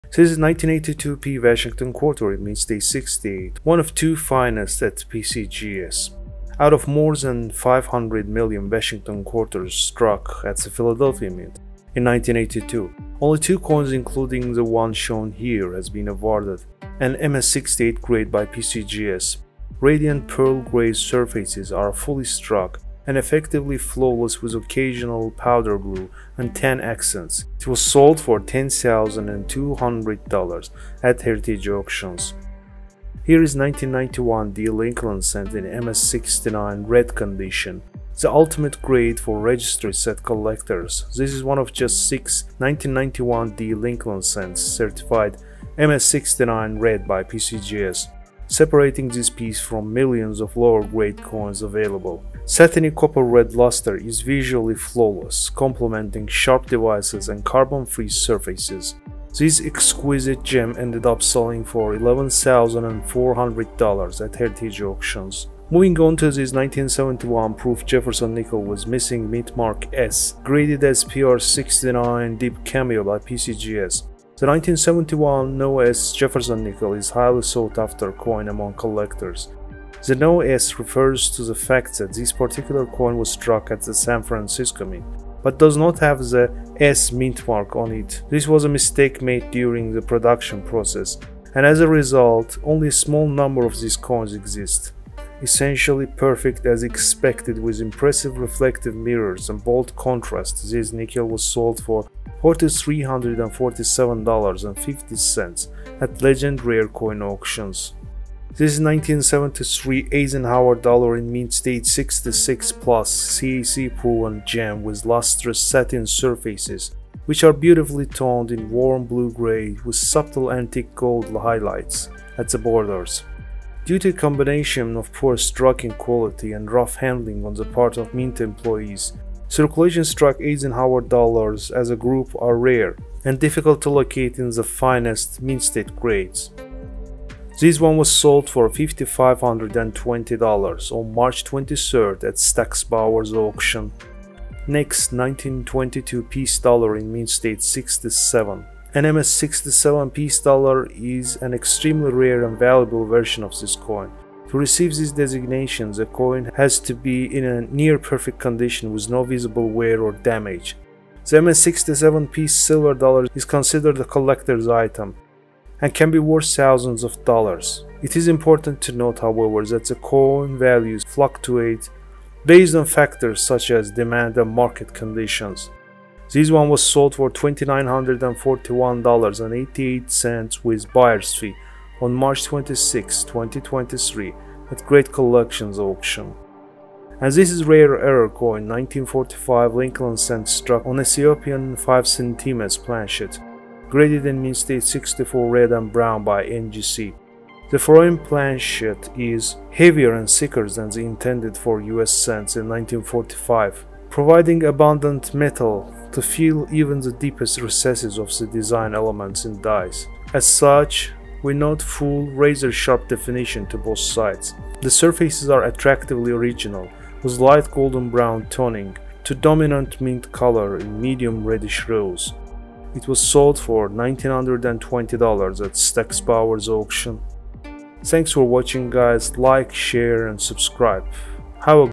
Shock! $10,200 per Coin! TOP 6 most expensive US Coins Sell in 2023!
Looking to invest in rare US coins? Check out the top 6 expensive 2023 listings in this video and start your collection today!